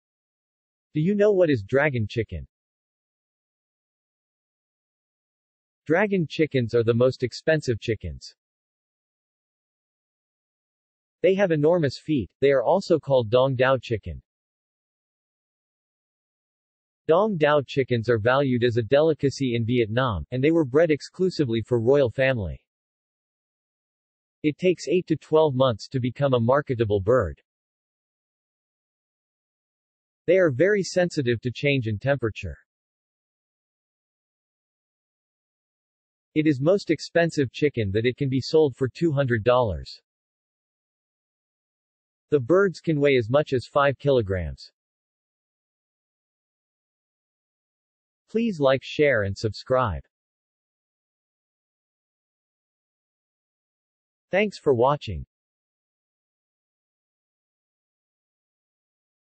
Amazing facts about animals | Amazing Facts about birds
in this we are giving knowledge about "dragon chicken" as they are very expensive birds "dragon chickens" are also called "dong Toa chicken" exclusively for royal family "dragon chicken" are very sensitive to changes in temperature and much much more